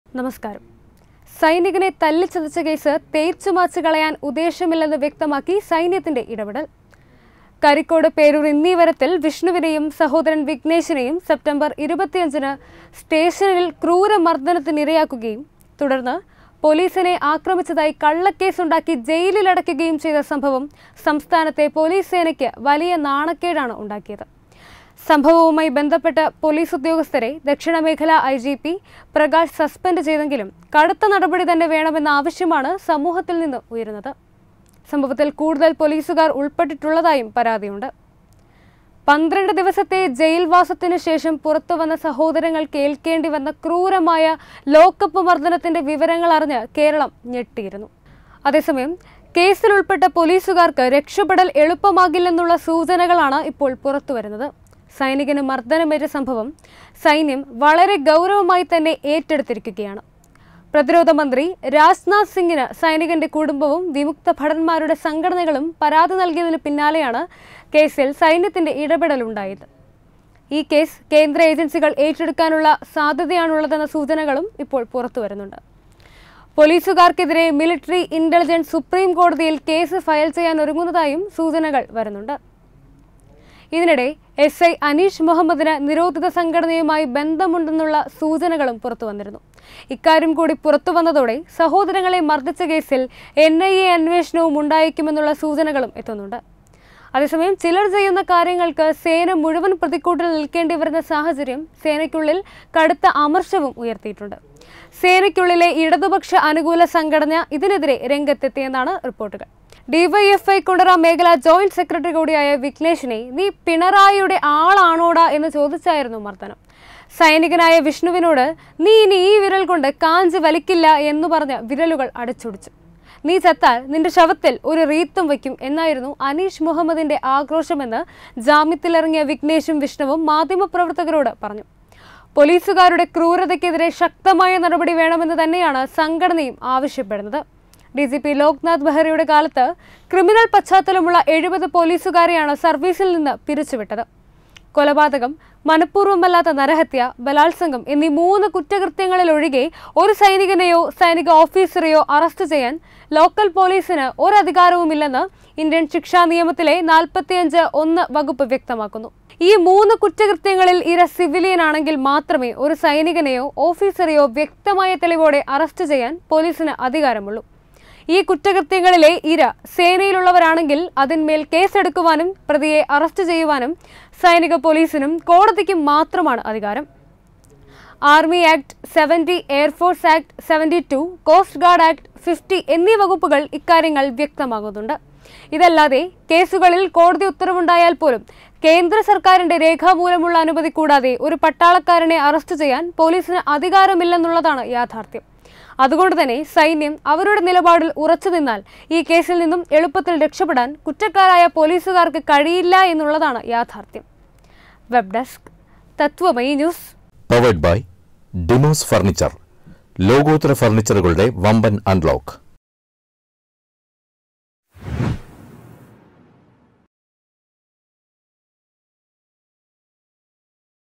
എസ്ഐ അനീഷ് മുഹമ്മദിന് നി-രോ-ധിത സംഘടനയുമായി ബന്ധമുണ്ടെന്നുള്ള സൂചനകളും പുറത്തു വന്നിരുന്നു
പോലീസുകാരന്റെ കേസ് അങ്ങനെയൊന്നും തീരില്ല സൈന്യം ഉറച്ചുതന്നെ | RAJNATH SING